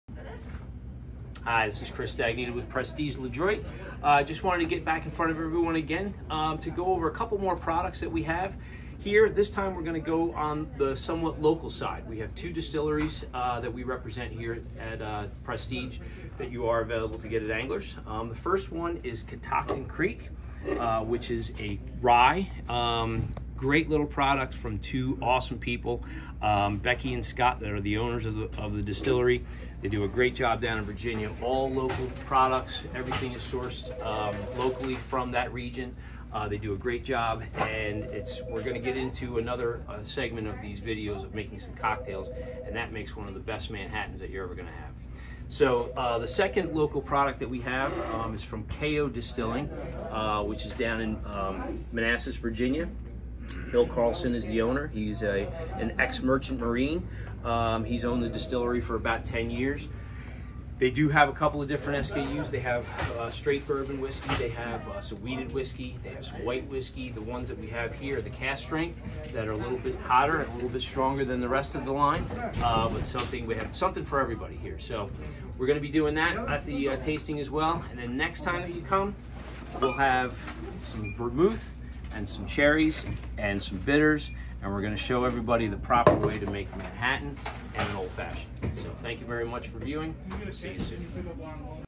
Local Whiskeys from Prestige-Ledroit
Chris from Prestige-Ledroit tells us about some of the locally distilled whiskeys that we carry in the Whiskey Cellar!